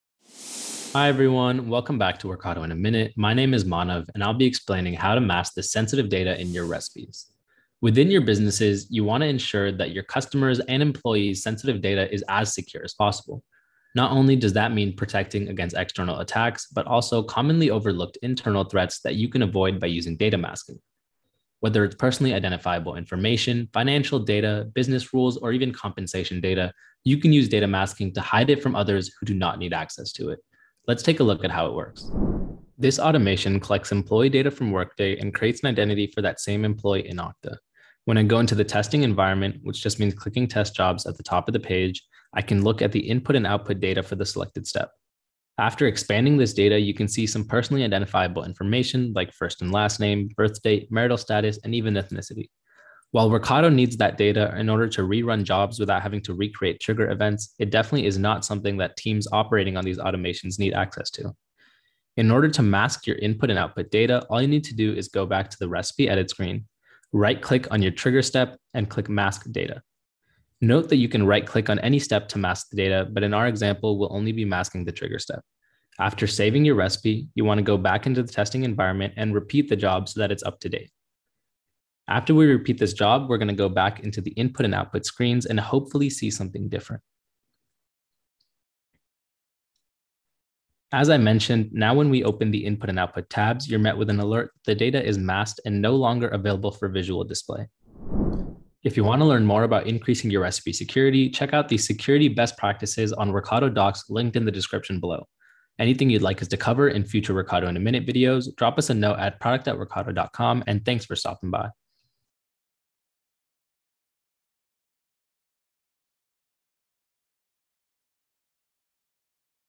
How to protect sensitive data with masking l Workato in a Minute
Within your businesses, you want to make sure your customers’ and employees’ sensitive data is as secure as possible. Not only does that mean protecting against external attacks, but also commonly-overlooked internal threats that we can avoid by using data masking.

Whether it’s personally identifiable information, financial data, business rules, or compensation data - you can use data masking to hide it from others who don’t need access to it.
Let’s see how this works.

0:00 Intro
0:32 Sample recipe and sensitive data
1:02 Masking input and output data
1:39 Outro